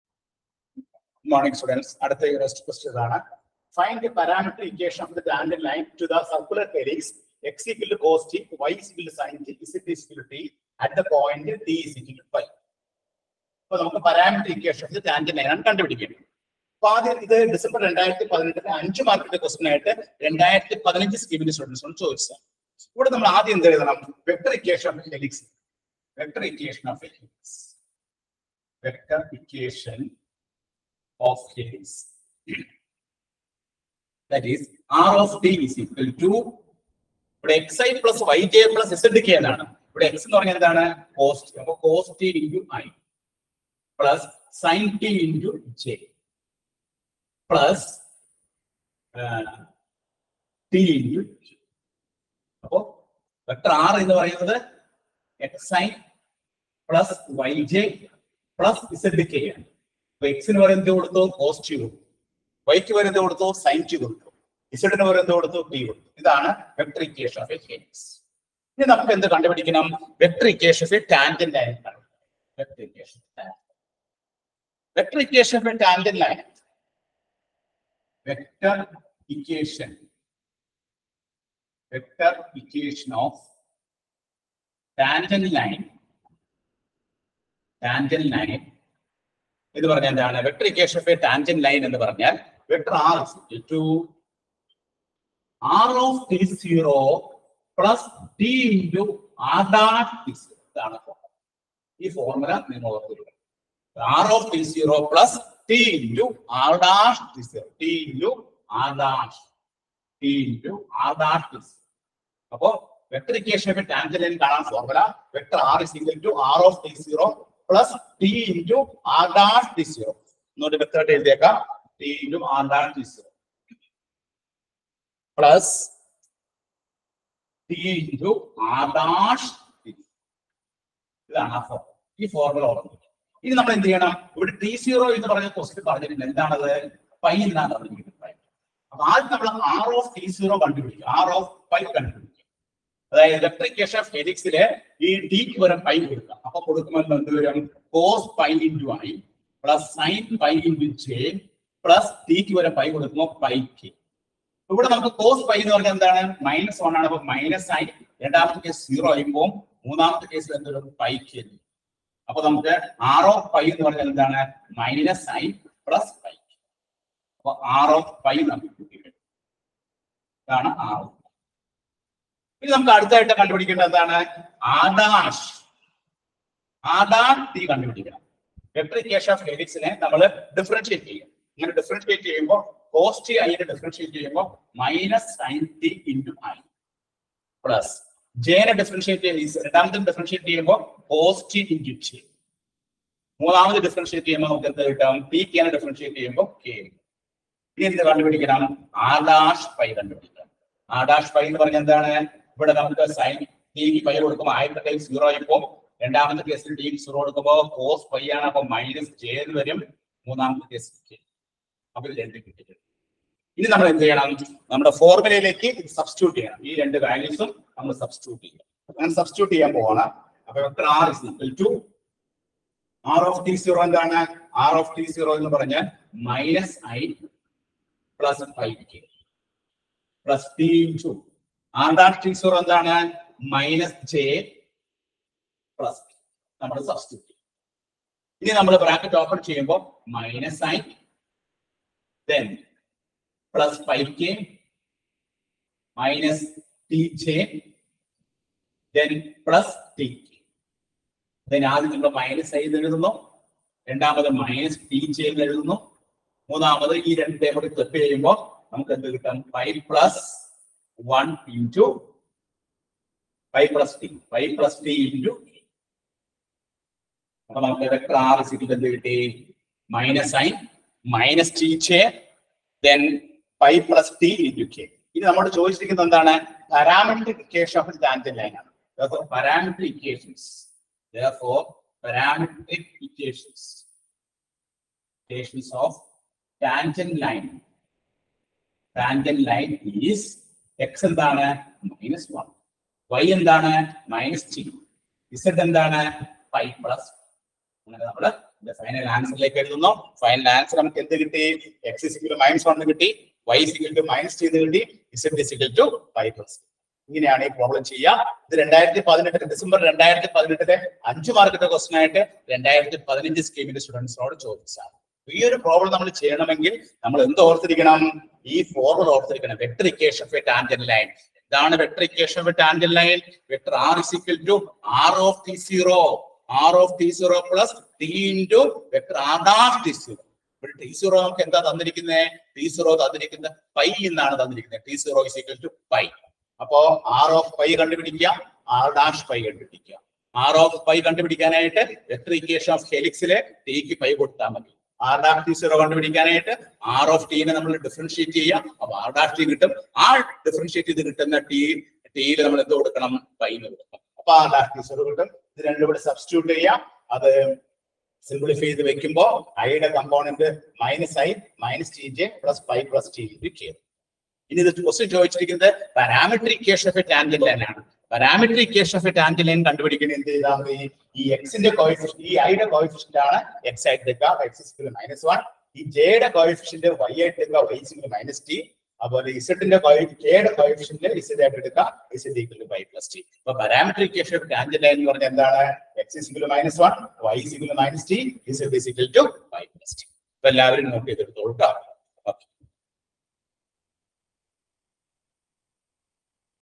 UNIVERSITY QUESTION-27 I MODULE -1| MAT 102 | 2019 SCHEME |B.TECH KTU | ENGINEERING MATHEMATICS
This video lecture of Calculus of Vector Functions contains concept of derivative of vector function, velocity ,speed, acceleration, gradient, directional derivative, divergence, curl and line integrals . This video will help you understand problems related to Calculus of Vector Functions.